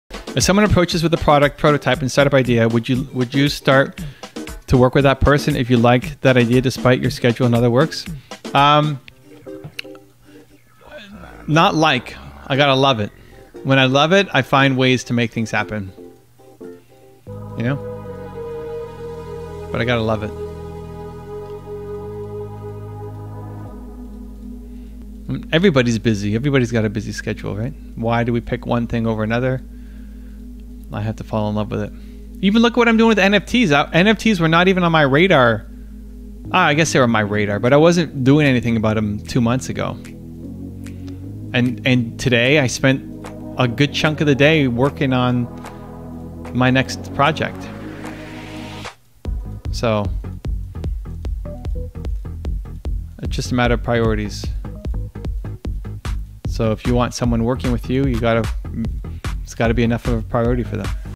Would You START Working With A Person Who APPROACH You With A Product PROTOTYPE?
In this video Evan answers this question: Would You START Working With A Person Who APPROACH You With A Product PROTOTYPE?

Here's the Automated Transcript:
00:00
if someone approaches with the product
00:01
prototype and set up idea would you
00:03
would you start
00:04
to work with that person if you like
00:06
that idea despite your schedule in other
00:07
works um
00:09
[Music]
00:11
not like i gotta love it when i love it
00:14
i find ways to make things happen
00:17
you know
00:17
[Music]
00:19
but i gotta love it
00:22
[Music]
00:25
everybody's busy everybody's got a busy
00:26
schedule right why do we pick one thing
00:28
over another
00:30
i have to fall in love with it even look
00:32
at what i'm doing with nfts nfts were
00:34
not even on my radar
00:36
i guess they were my radar but i wasn't
00:38
doing anything about them two months ago
00:41
and and today i spent a good chunk of
00:44
the day working on
00:46
my next project
00:49
so
00:51
[Music]
00:52
it's just a matter of priorities
00:56
so if you want someone working with you
00:57
you gotta
00:59
it's got to be enough of a priority for
01:00
them